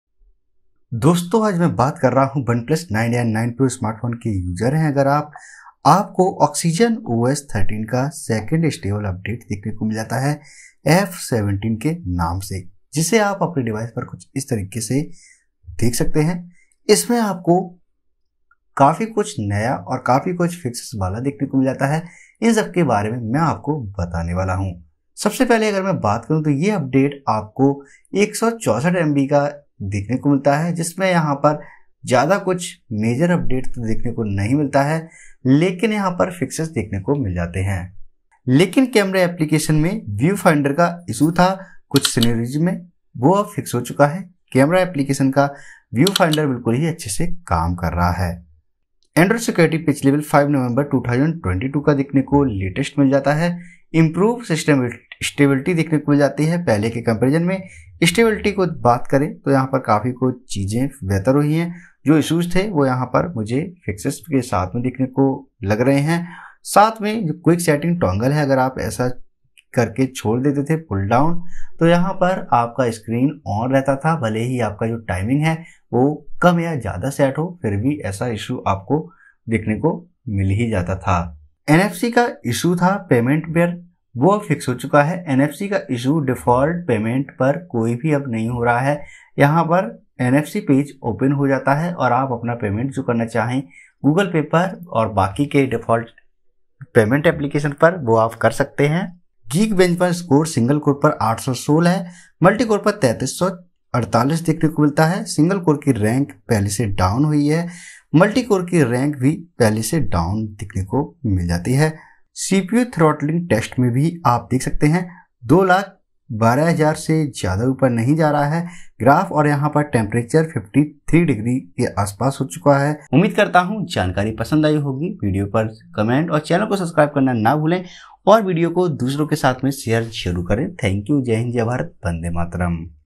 OnePlus 9 Oxygen OS 13 "F.17" Review - The Best OnePlus Phone Yet!
In this video, I'm going to be doing a deep review of OnePlus 9 Oxygen OS 13 "F.17". This new Oxygen OS update brings some big changes, and I'll be showing you everything that's new!

This Oxygen OS update is huge, and it's definitely worth installing if you own a OnePlus 9 or 9 Pro. I'll be going over everything that's new, including the new design, updated features, bug fixes, and performance improvements. Make sure to check out this video to learn everything you need to know about this latest Oxygen OS update!